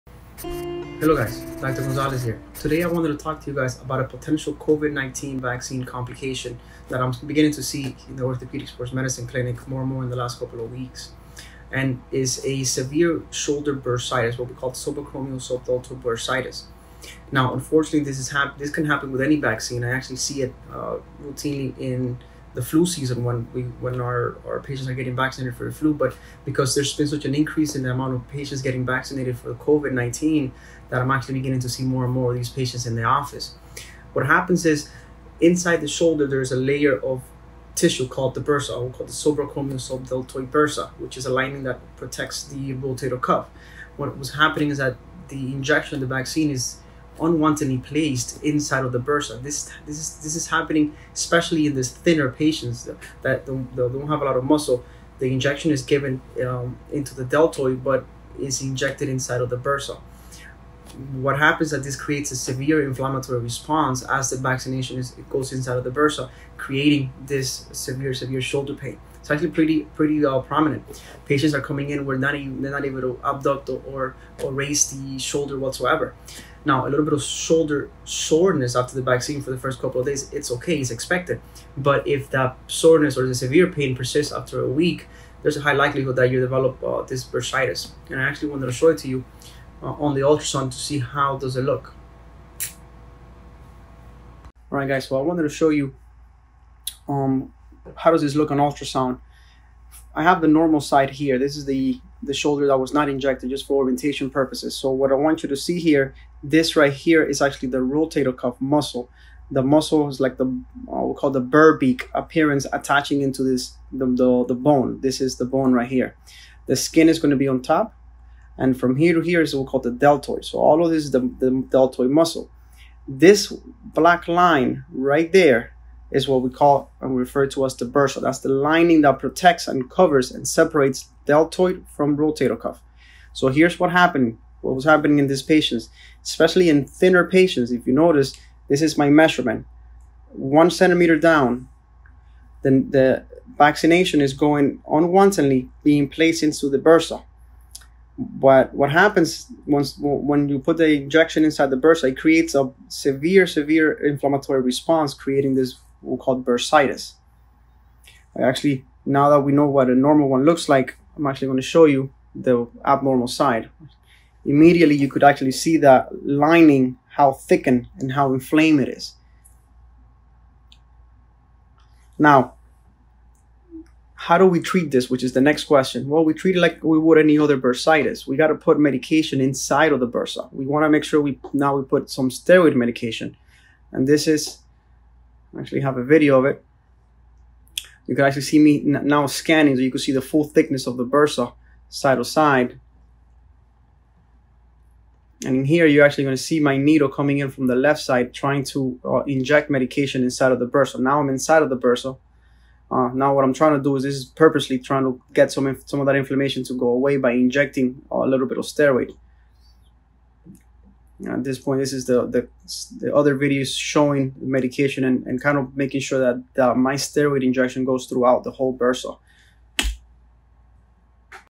COVID-19 vaccine shoulder complication.
I am beginning to see this more and more as the numbers of vaccinated patients grow.

Shoulder Bursitis after the COVID-19 vaccines. Happens more in thinner patients. The vaccine is mistakenly injected inside the subacromial-subdeltoid bursa.  This leads to a severe localized inflammatory response in the bursa, creating severe shoulder pain.

Symptoms: Severe shoulder pain with any movement, specially overhead. Difficulty sleeping and associated with some weakness. This can last from weeks to months if left untreated.

If you received the COVID-19 vaccine and have developed severe debilitating shoulder pain, there is a possibility that the injection was placed in the bursa and unfortunately you have develop this complication. You may need to see a specialist to confirm this problem and treated as soon as possible.